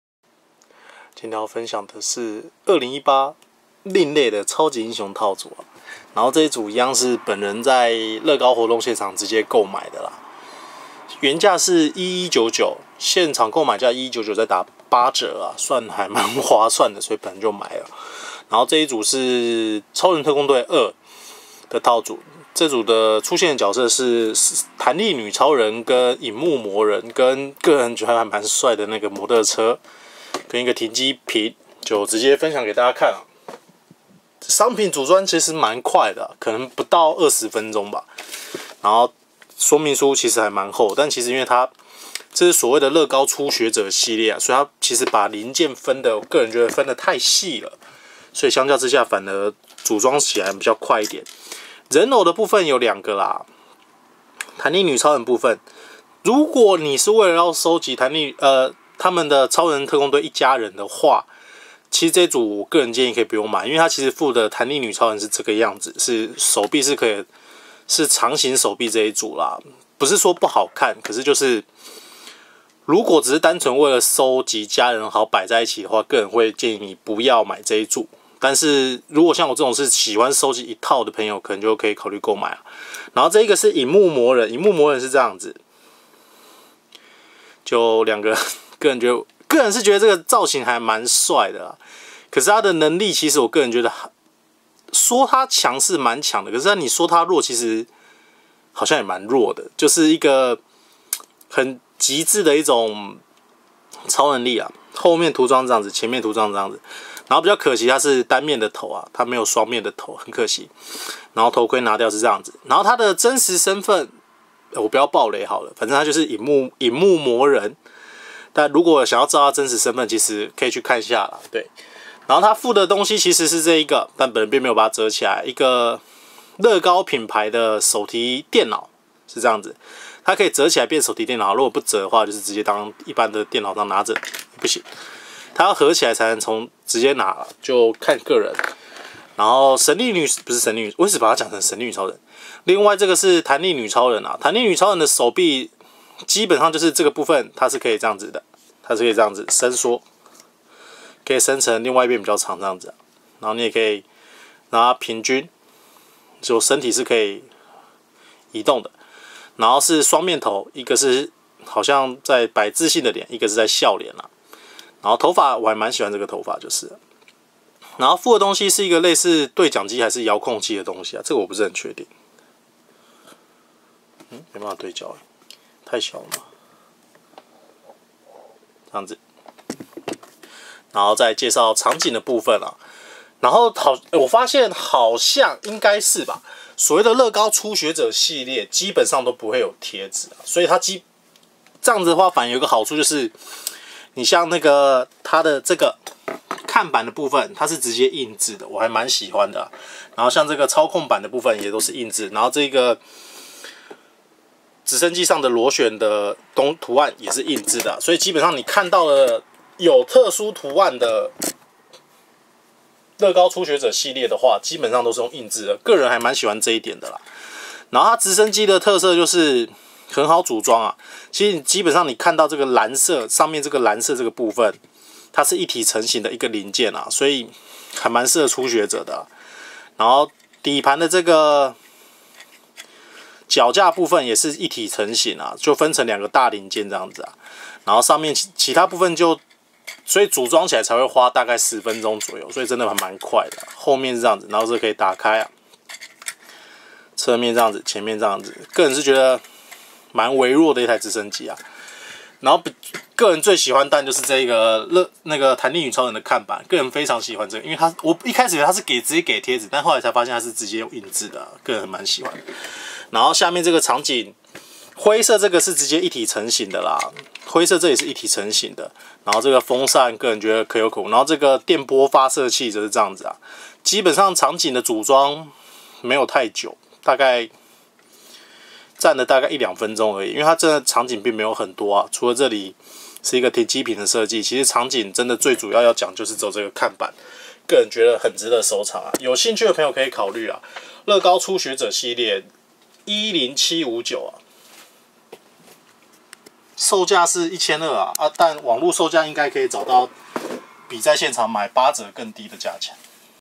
豪宅玩具~1102~2018 LEGO 10759 樂高Juniors Incredibles 2超人特攻隊2 彈力女超人Elastigirl螢幕魔人
可惜這次出版的三款超人特攻隊2的套組都沒有衣夫人跟酷冰俠的人偶!
意外的發現~樂高Juniors~系列~沒有貼紙!它有需要塗裝的部分都是用印製的!超級棒的!之前怎麼都沒有發現這系列有這個特點!